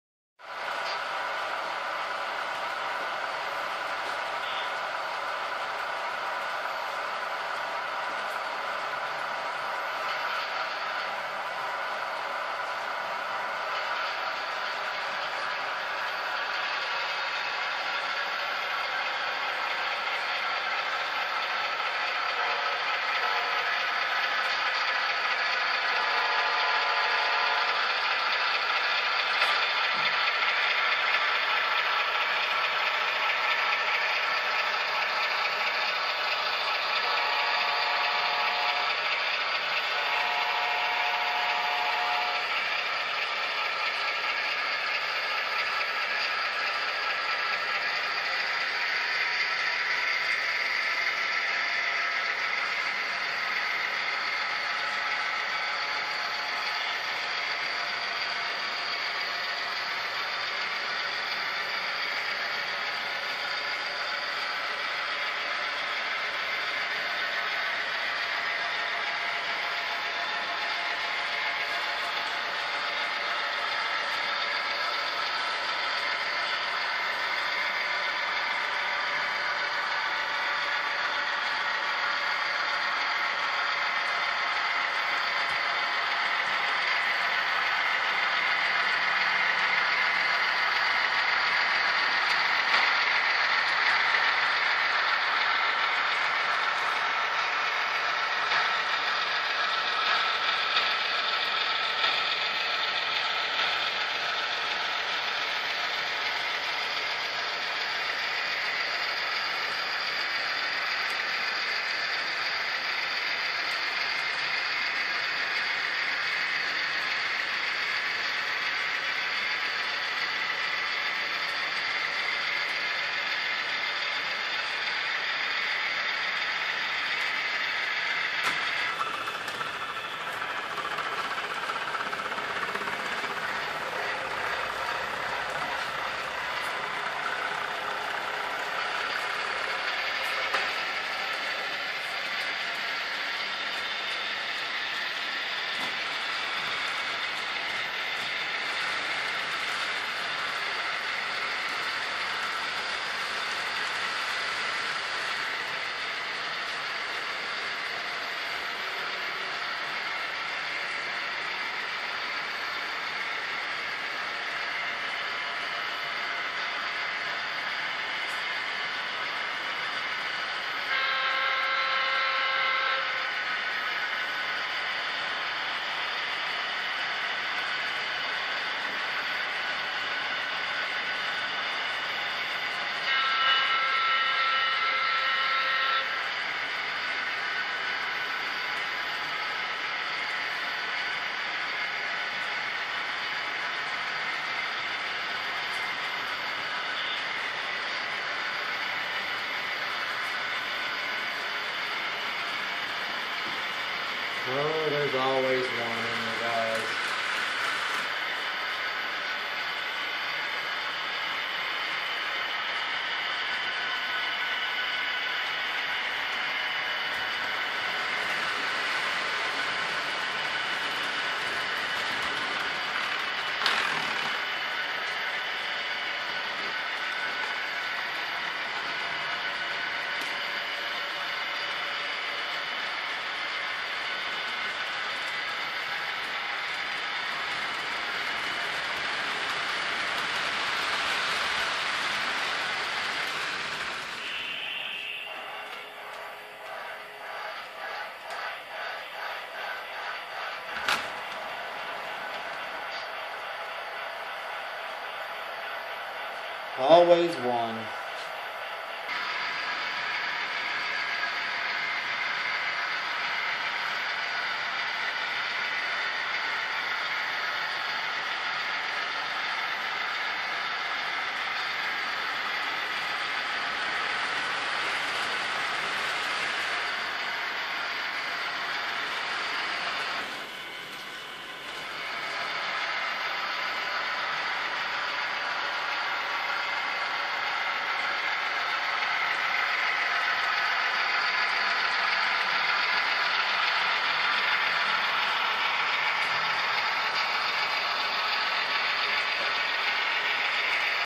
ICG heavy with some modern stuff thrown on for fun
Well, after working 28 days straight thought I would make a short vid. Damn Murphy's Law was running all kinda rampant down in the train room. Jeesh.